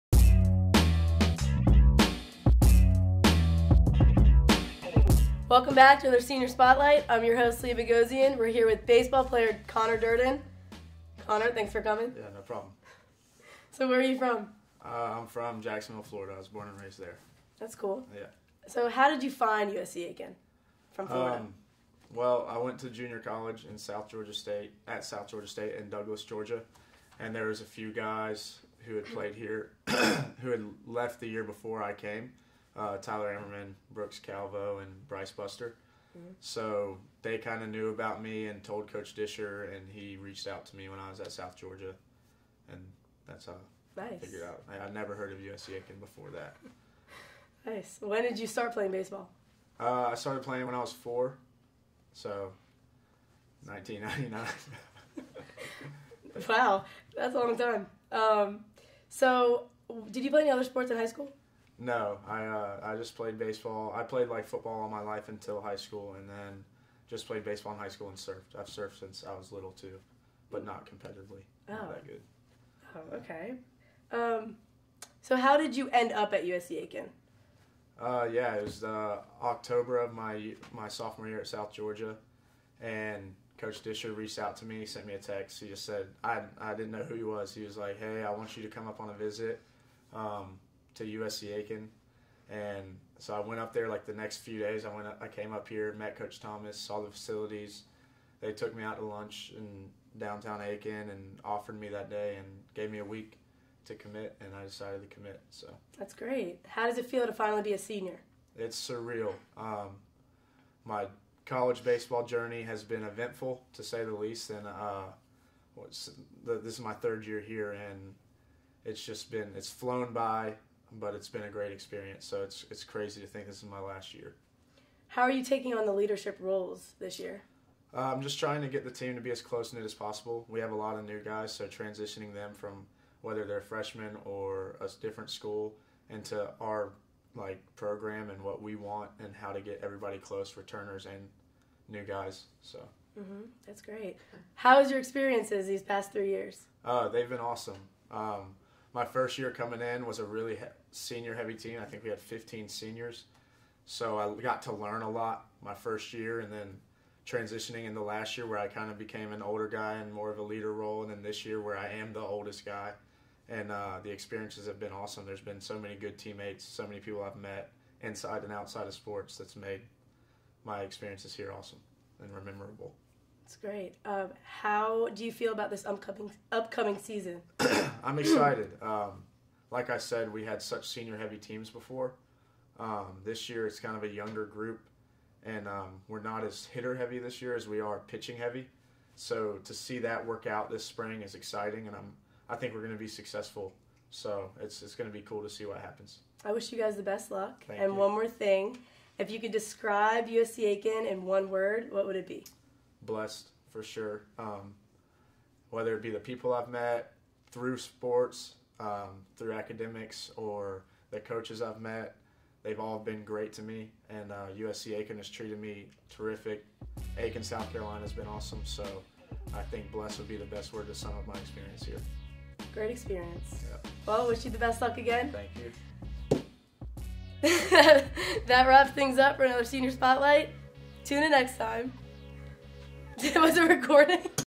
Senior Spotlight: Conner Durden
We sat down with PacerBSB player Conner Durden to find out more about his time at USC Aiken